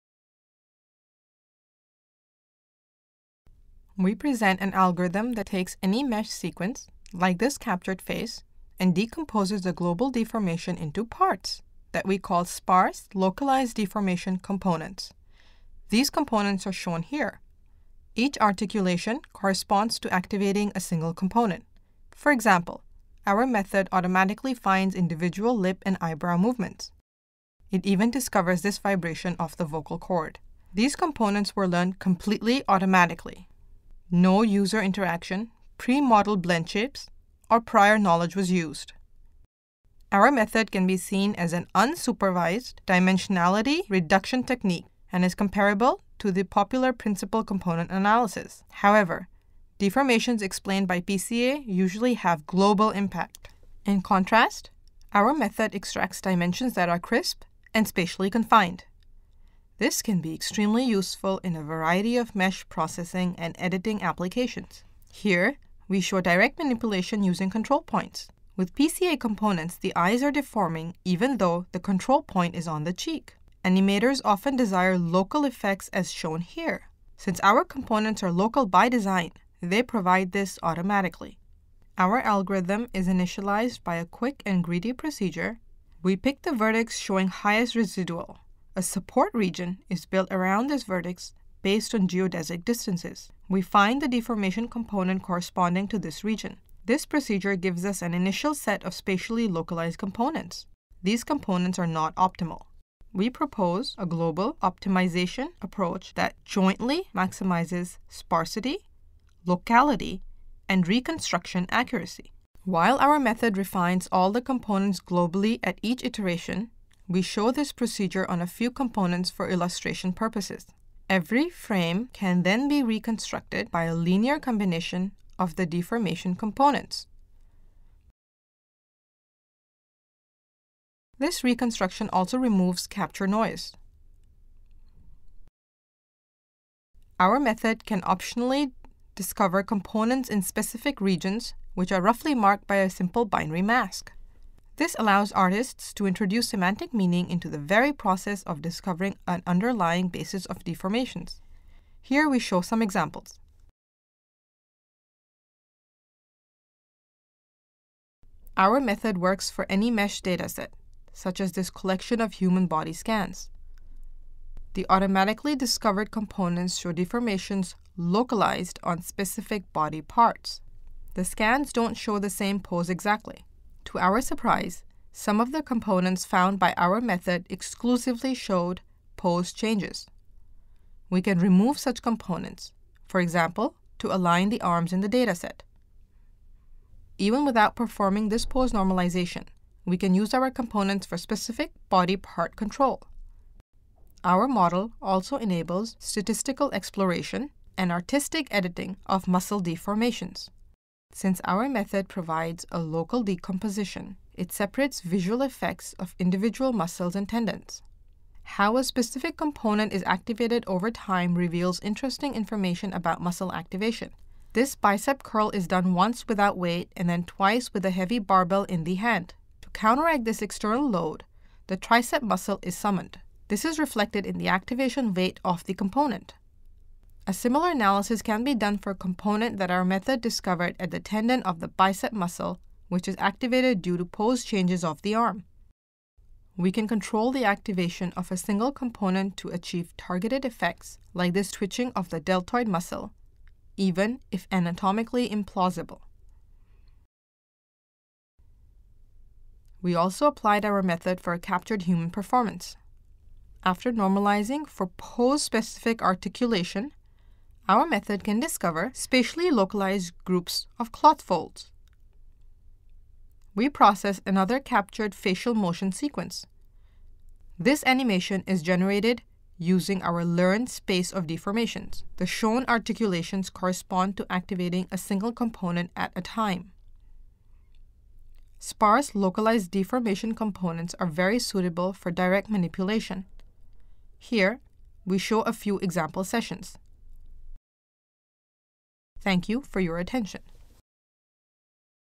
Sparse Localized Deformation Components (SIGGRAPH Asia 2013)
Thomas Neumann, Kiran Varanasi, Stephan Wenger, Markus Wacker, Marcus Magnor, and Christian Theobalt: "Sparse Localized Deformation Components", conditionally accepted to SIGGRAPH Asia 2013.

Abstract:
We propose a method that extracts sparse and spatially localized deformation modes from an animated mesh sequence. To this end, we propose a new way to extend the theory of sparse matrix decompositions to 3D mesh sequence processing, and further contribute with an automatic way to ensure spatial locality of the decomposition in a new optimization framework. The extracted dimensions often have an intuitive and clear interpretable meaning. Our method optionally accepts user-constraints to guide the process of discovering the underlying latent deformation space. The capabilities of our efficient, versatile, and easy-to-implement method are extensively demonstrated on a variety of data sets and application contexts. We demonstrate its power for user friendly intuitive editing of captured mesh animations, such as faces, full body motion, cloth animations, and muscle deformations. We further show its benefit for statistical geometry processing and biomechanically meaningful animation editing. It is further shown qualitatively and quantitatively that our method outperforms other unsupervised decomposition methods and other animation parameterization approaches in the above use cases.